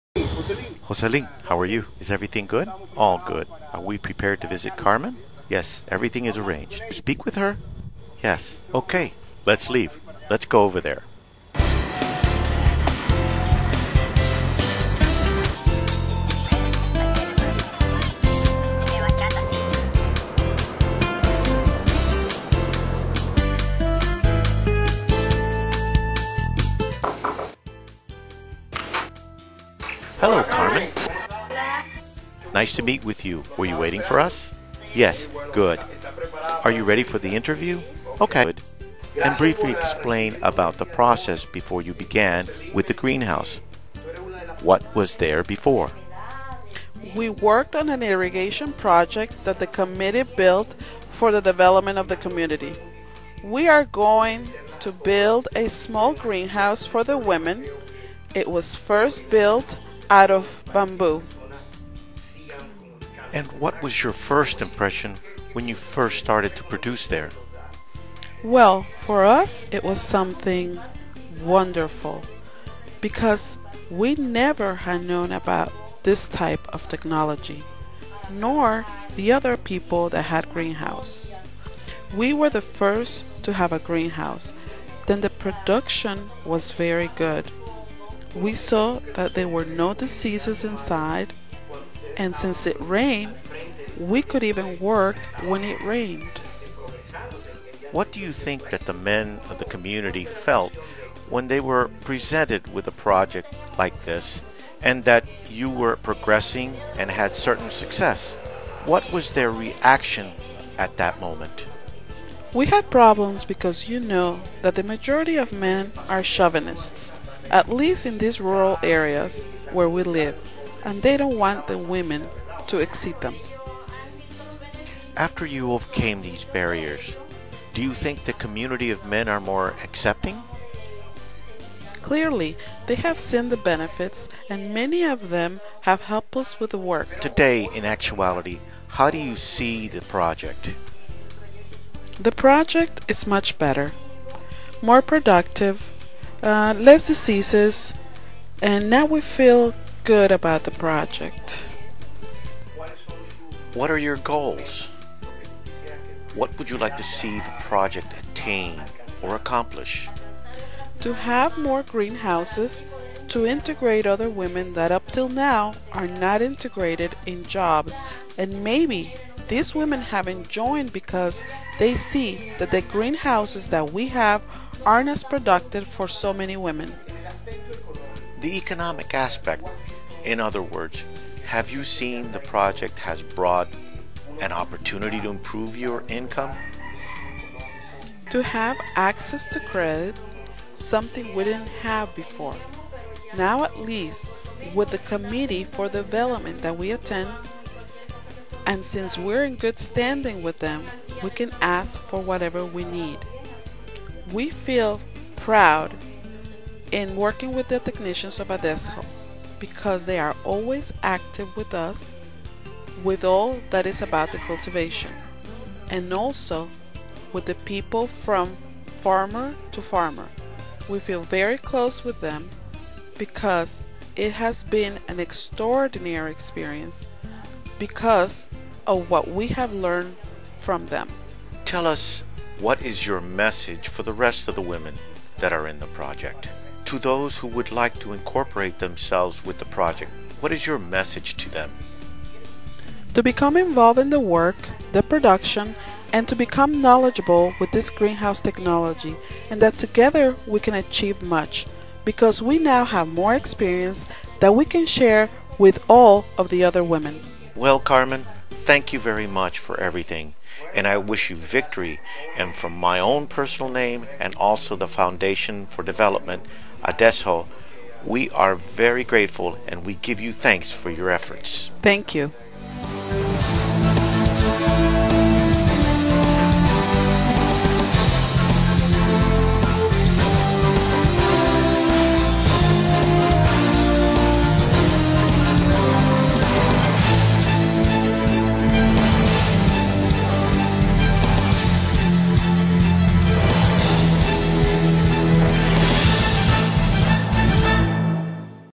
San Jose de Ocoa
Re-edited version of the San Jose de Ocoa greenhouse project in Santo Domingo.
This version of the original video is an English voiceover/translation from the original Spanish language version.